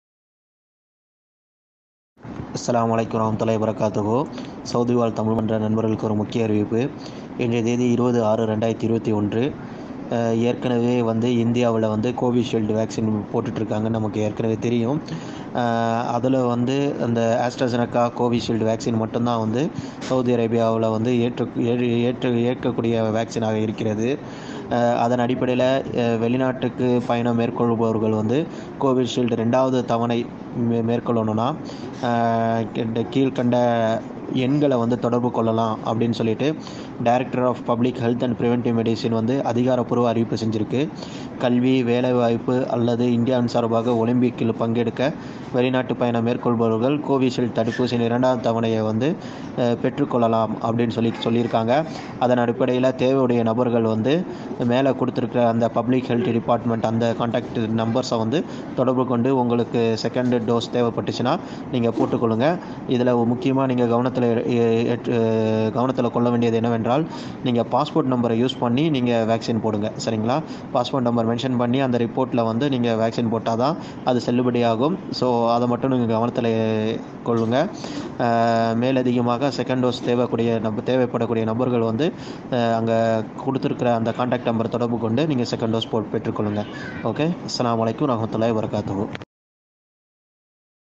சவுதி அரேபியாவில் இருப்பவர்கள் மற்றும் சவுதி அரேபியா வருபவர்களுக்கு முக்கிய தகவல்
*கல்வி, வேலைவாய்ப்பு அல்லது இந்தியாவின் சார்பாக ஒலிம்பிக்கில் பங்கெடுக்க வெளி நாட்டுப்பயணம் மேற்கொள்ளுபவர்கள் கோவிஷீல்ட் COVISHIELD தடுப்பூசியின் 2வது தவணைக்கு கீழ்கண்ட நபர்களை தொடர்புகொள்ளவும்.*
*இது Directorate of Public Health & Preventive Medicine அதிகாரப்பூர்வ அறிவிப்பு.*

கோவின் தளத்திலும் இவ்வசதி இயக்கப்பட்டுள்ளது.

Contacts in TN for 2nd dose of Covishield vaccine to undertake International Travel for Education/ Employment or as part of India’s contingent for Olympics Facility has also been enabled in CowinPorta